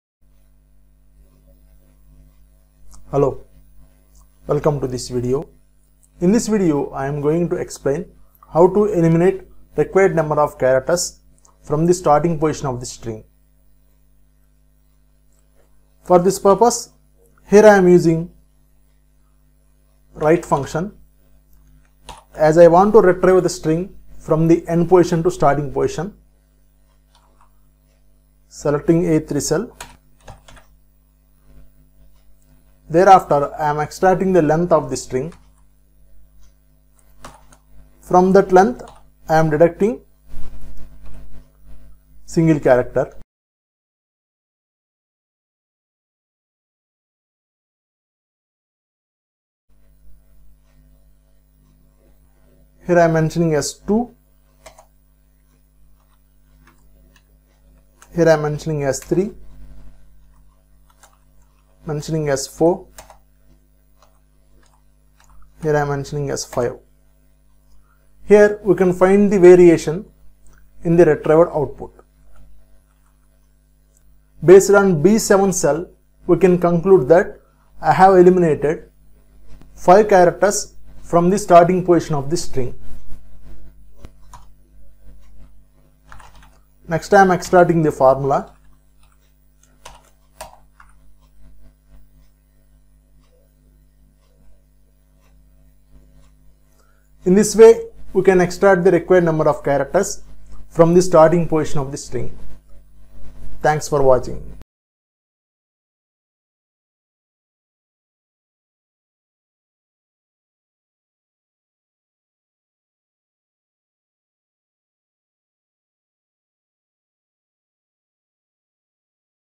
Excel Functions||How To Eliminate required Number Of Characters From Start Position To End Position
Excel Functions||How To Eliminate required Number Of Characters From Start Position To End Position of The String